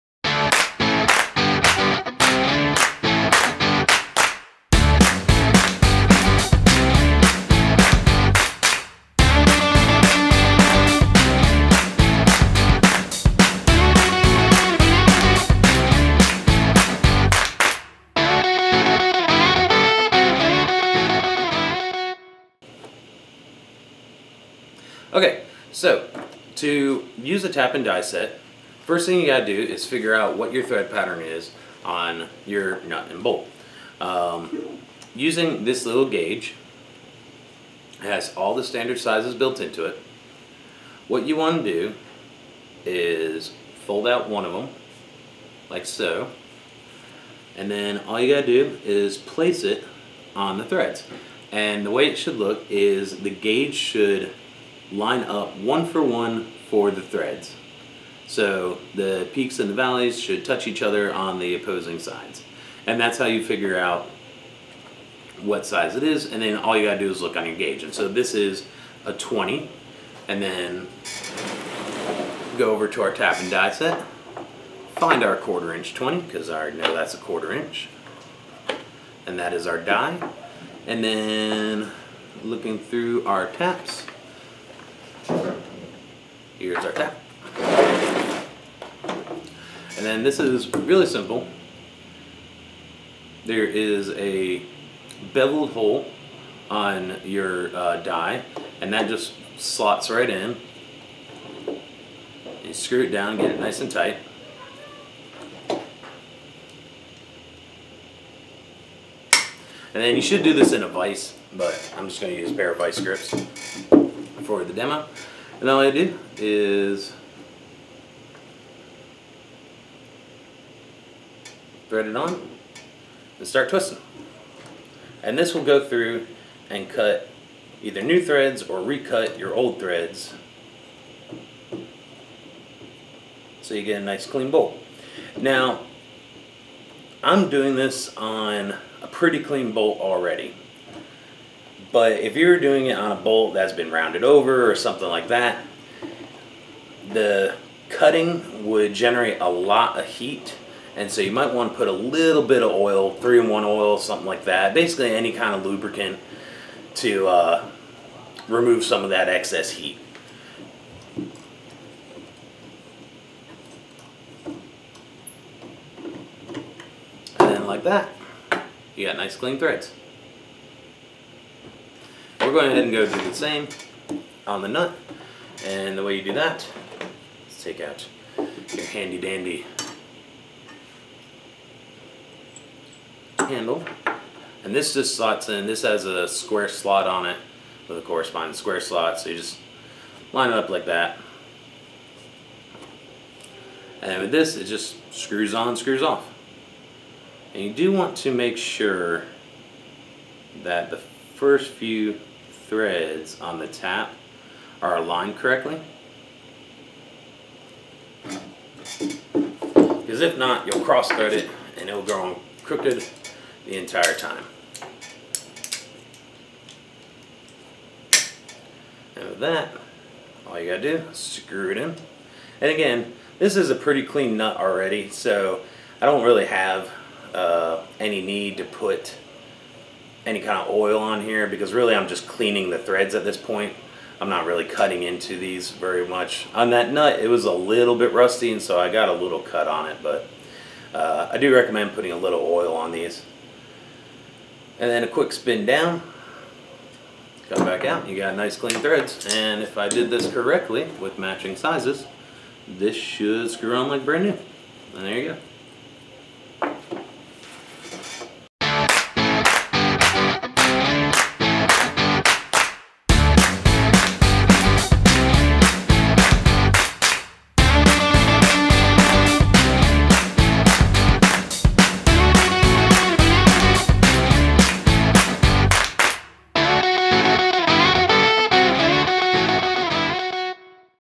[39] - How to do stuff - How to use a Tap and Die set
In this episode we will go over the basics of How to use a Tap and Die set